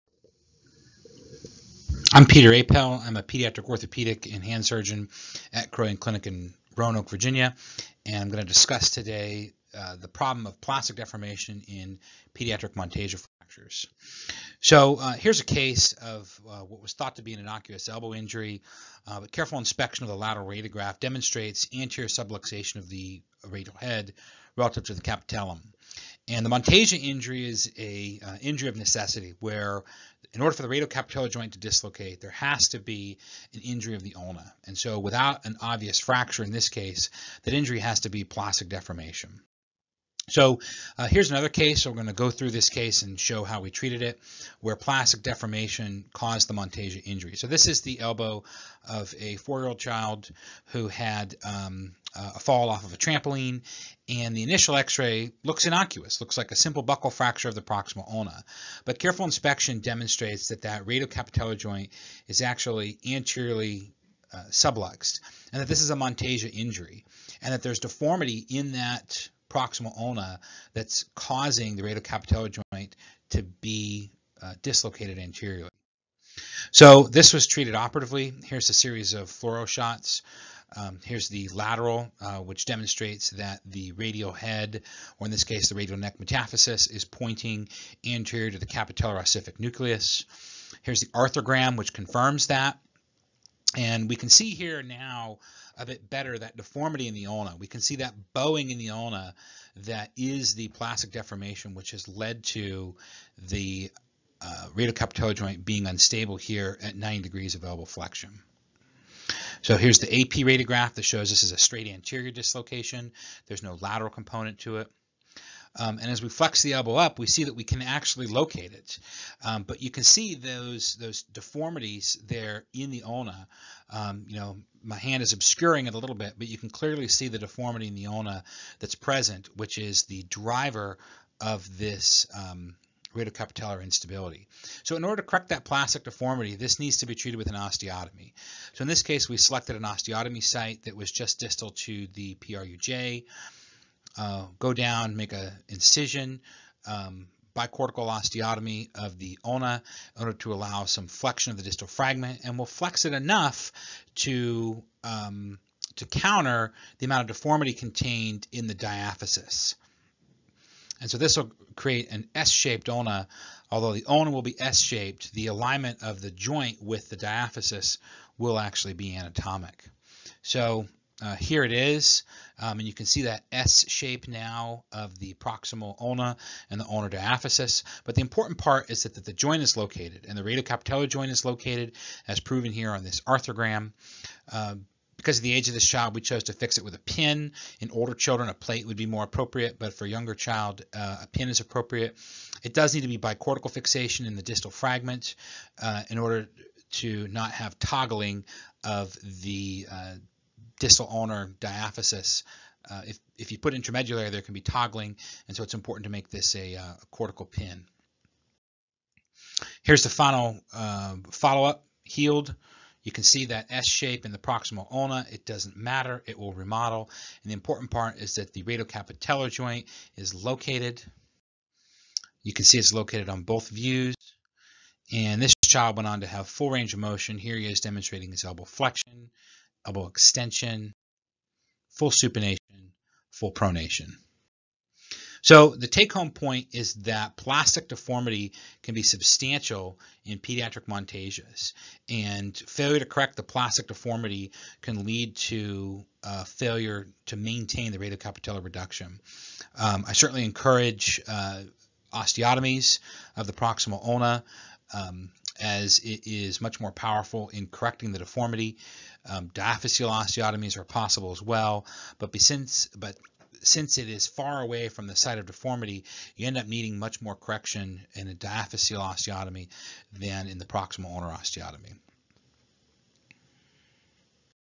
Plastic Deformation in Pediatric Monteggia Fractures
A brief description and solution for plastic deformity in pediatric Monteggia fractures.